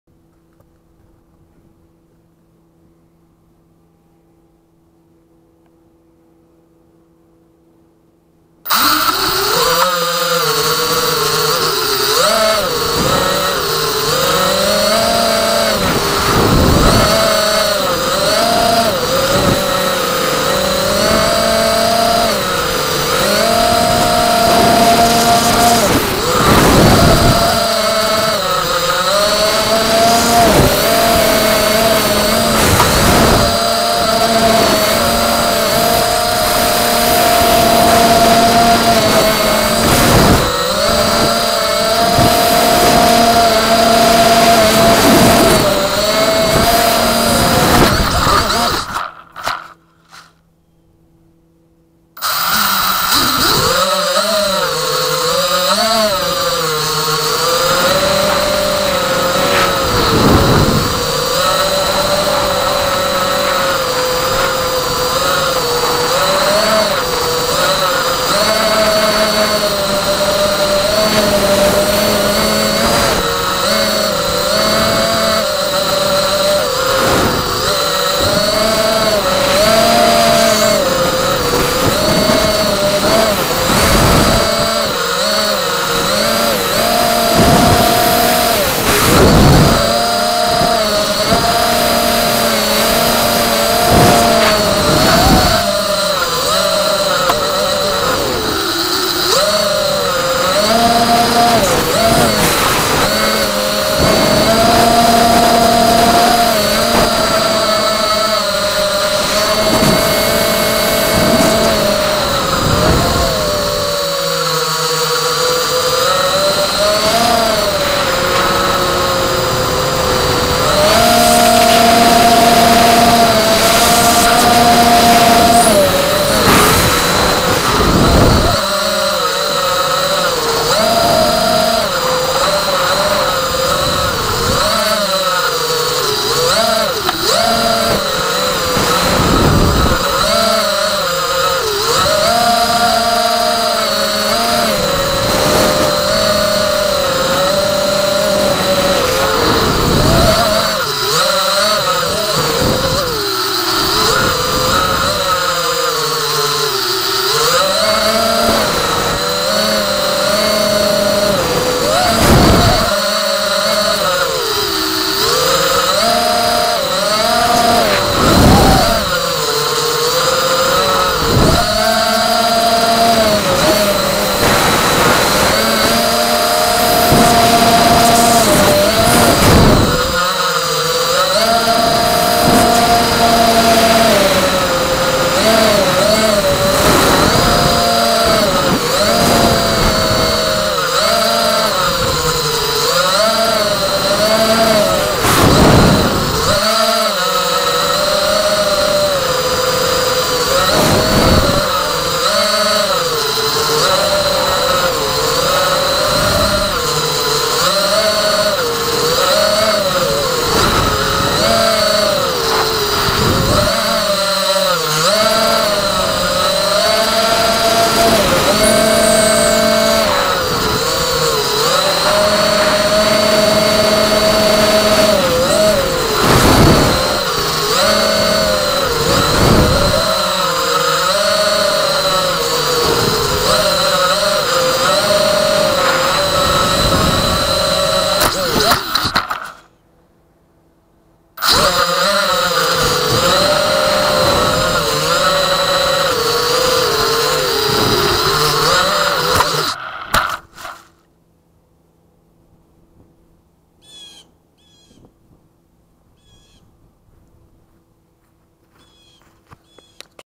Testing out the Waterproofness of the Winter quadcopter
280 quadcopter